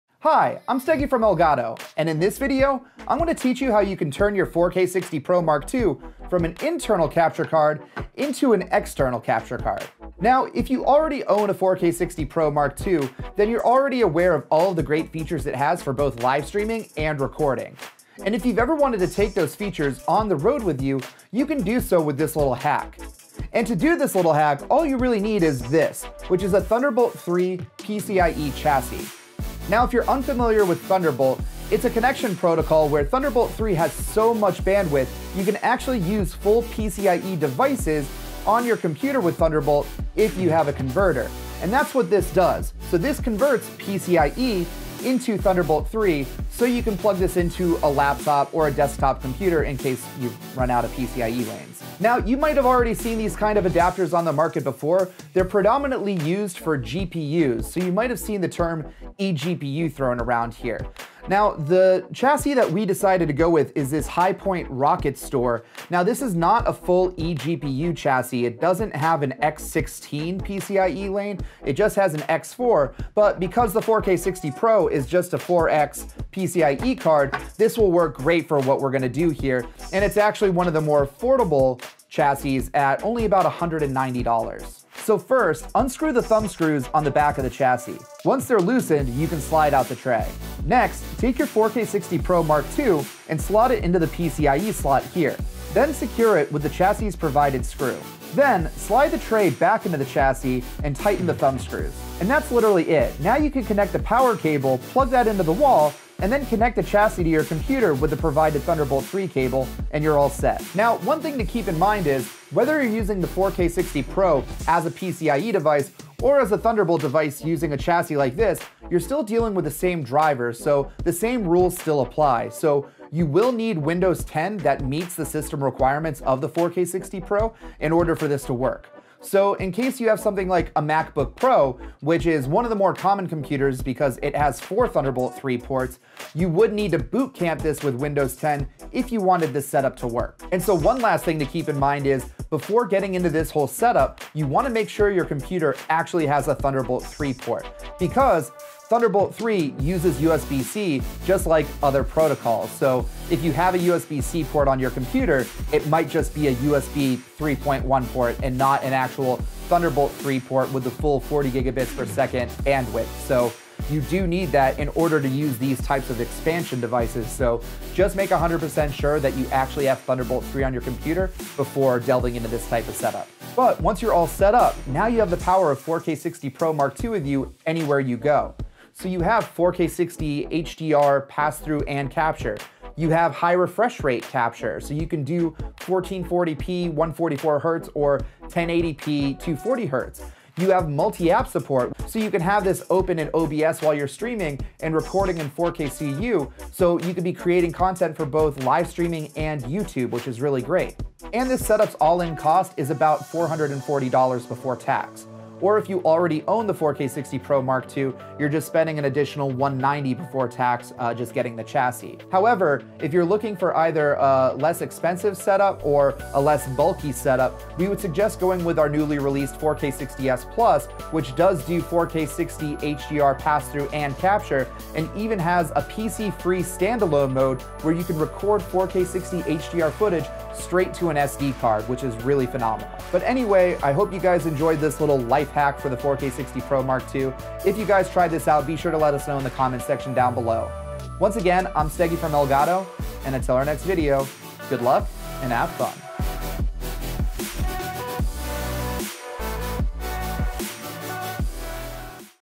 How to Turn Elgato 4K60 Pro into an External Capture Card
In this video, we show you how you can easily turn your 4K60 Pro MK.2 into an external Thunderbolt 3 capture device.

► HighPoint RocketStor